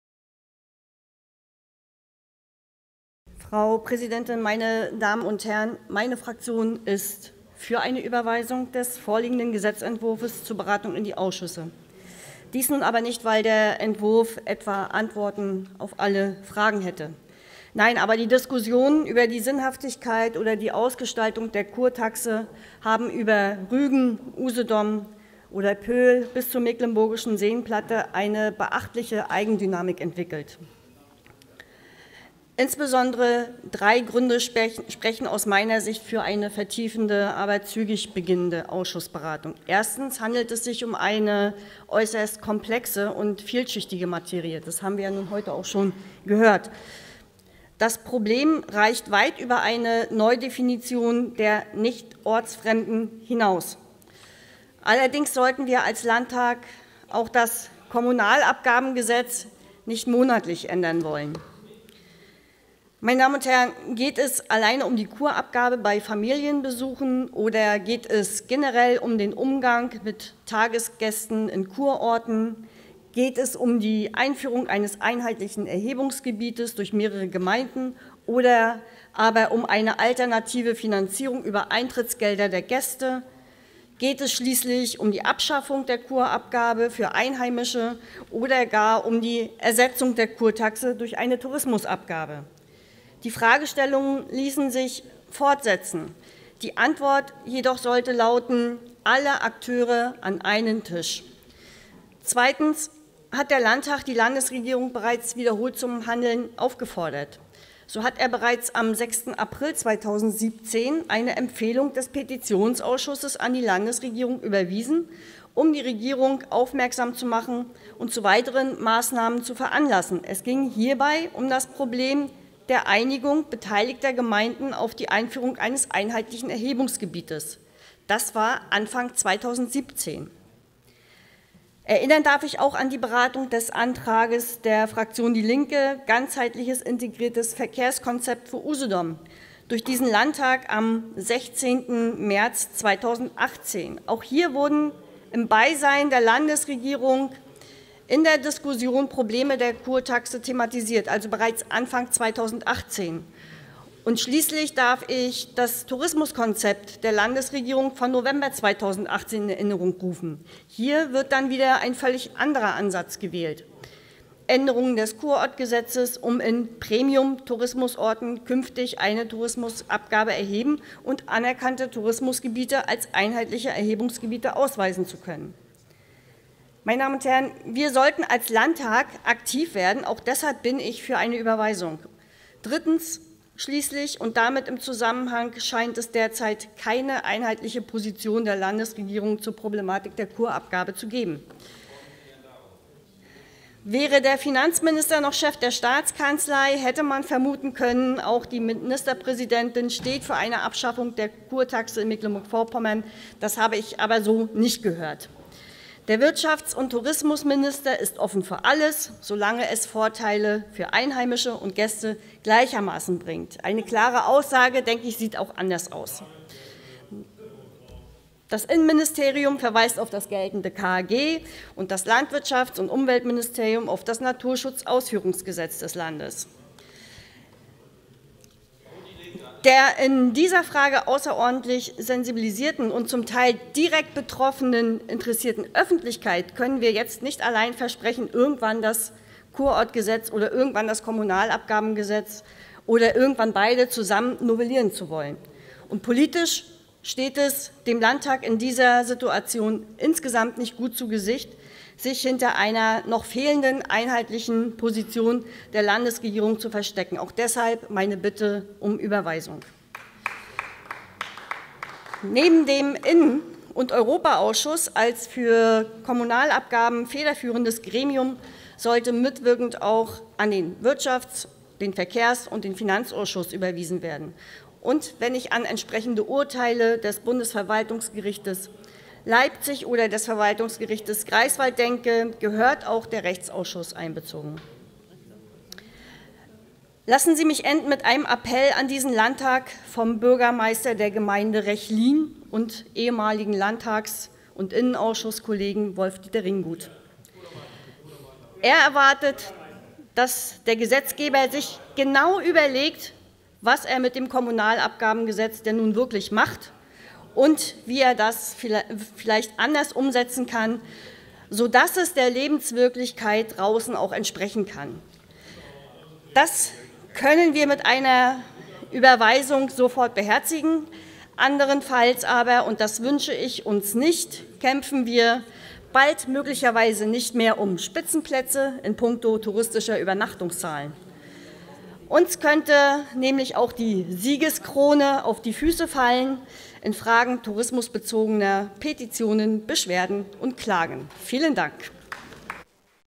Gesetz zur Änderung des Kommunalabgabengesetzes
Rede
Jeannine Rösler